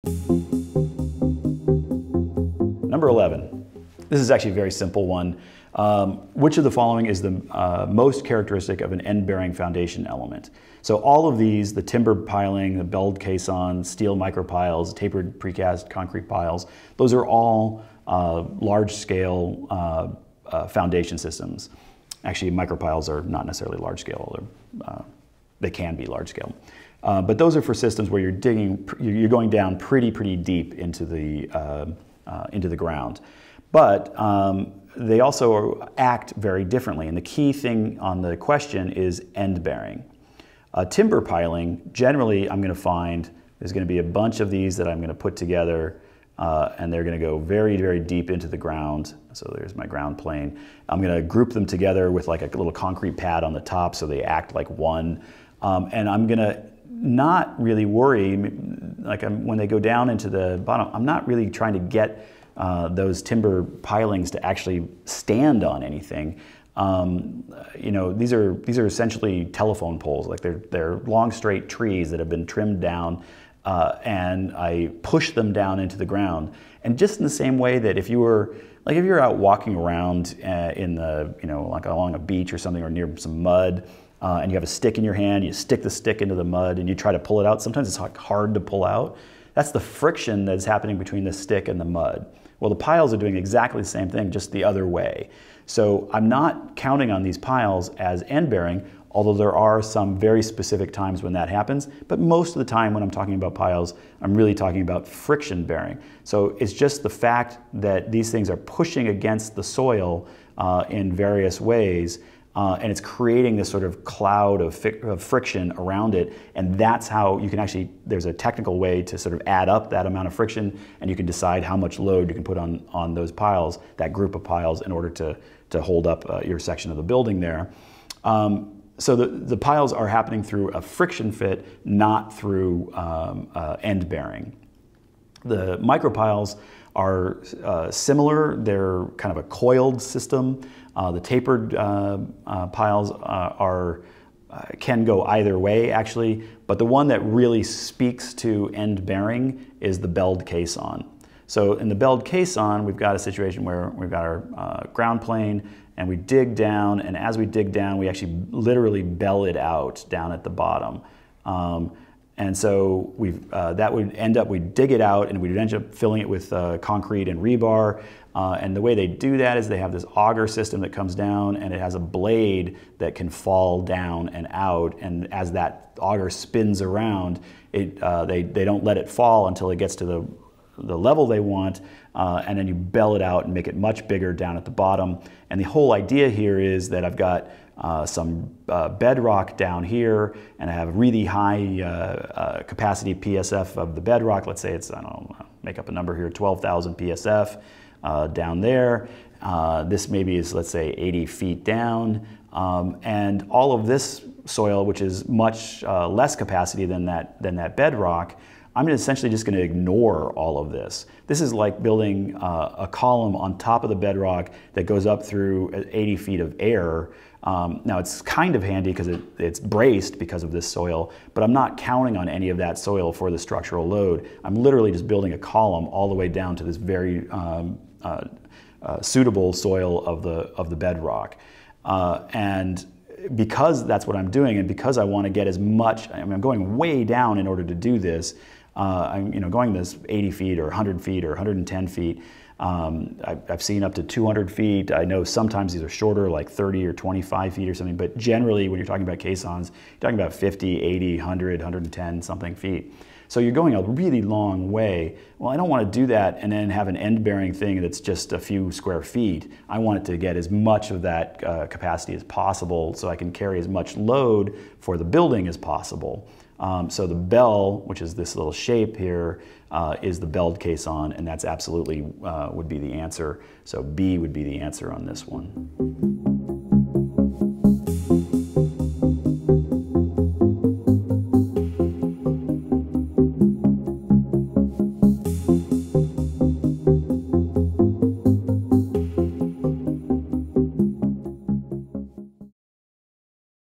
End-Bearing Foundation - Building Design and Construction Mock Exam - Architect Registration Exam
This webinar will focus on reviewing mock exam questions for the Building Design and Construction exam. Prior to the review, we sent out an exam to all those that signed up, gave some time to take the mini-exam, and then went through each question one by one during the online / live session, reviewing the answers, strategies, and resources for how to answer them.

In this section, Mike Newman discusses a question from the Building Design and Construction exam that asks which of the following is MOST characteristic of an end-bearing foundation element? It then lists four possible options: timber piling, belled caisson, steel micropile, or tapered precast concrete pile. Mike will go over each answer, reveal the correct one and how he got to that answer.